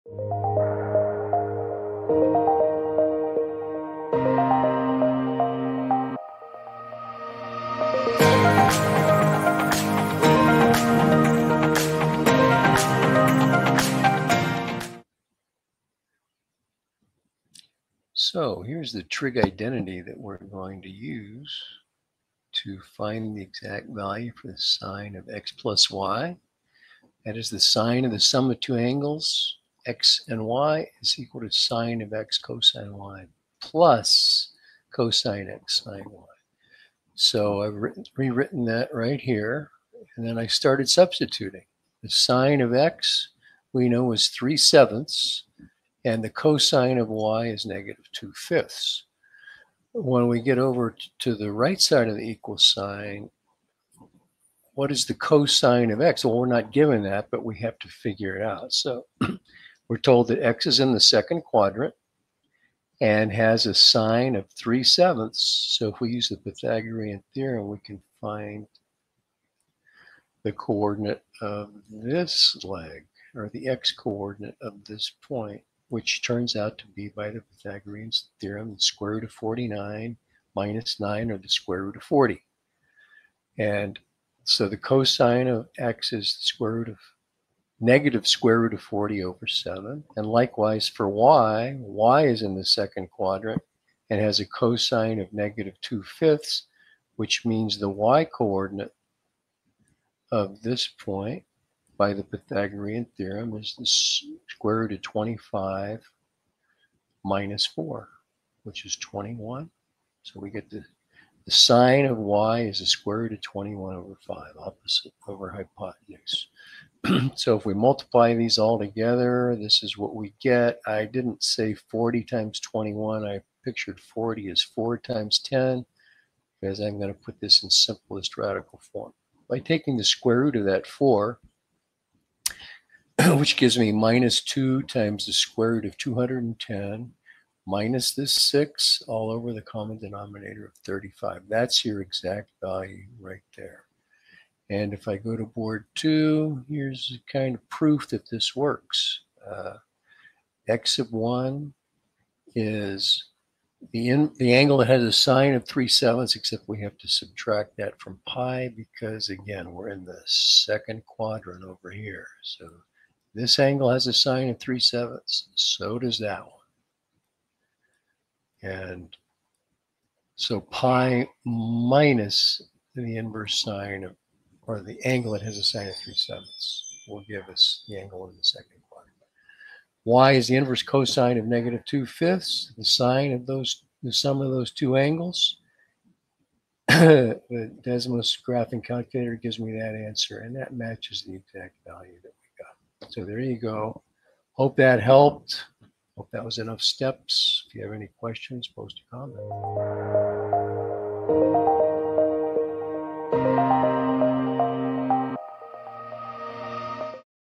Given sin x and cos y find exact value for sin(x+y)
Question: Given sin(x)=3/7 where (pi)/2
Please show as many steps as possible

Answered By:

Doug C.